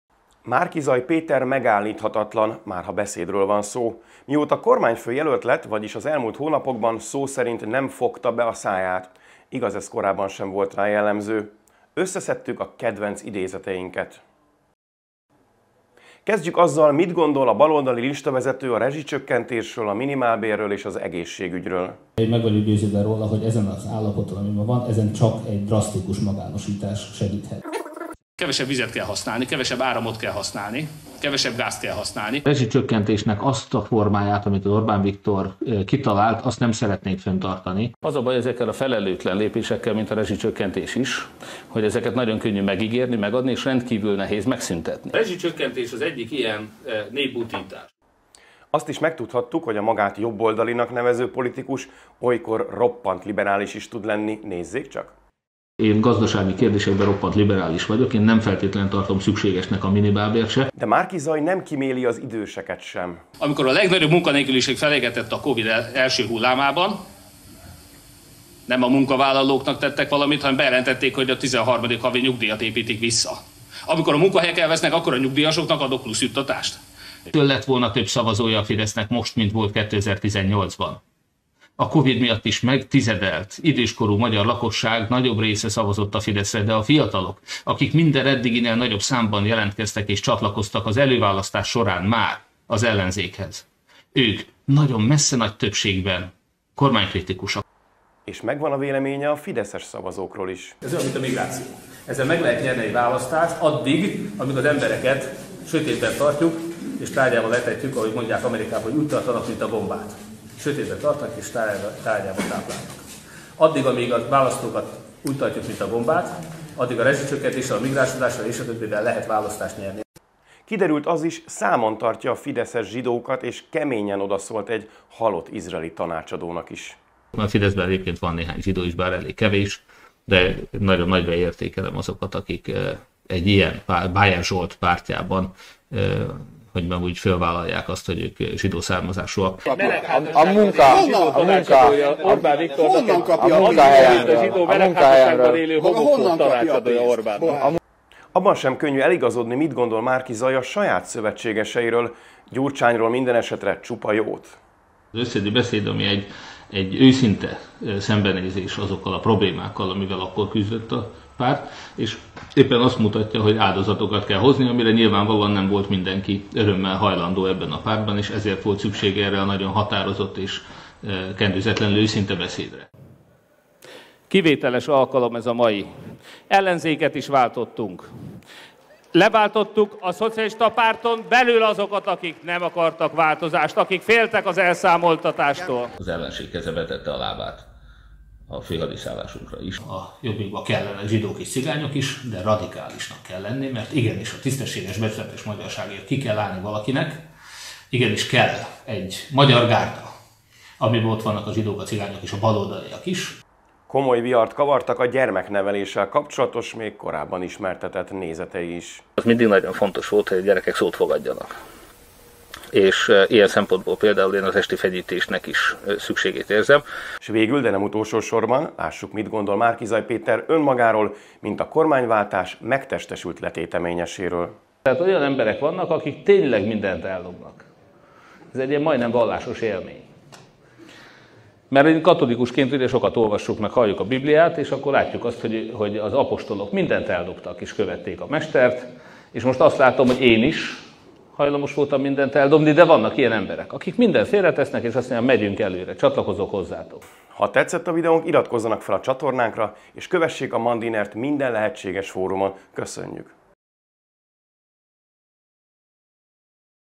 Roppant liberális, Gyurcsányt méltatja és Jézushoz hasonlítja magát – Márki-Zay legerősebb mondatai
A Mandiner Reakció legújabb részében a baloldal miniszterelnök-jelöltjének „aranyköpéseit” szedtük csokorba.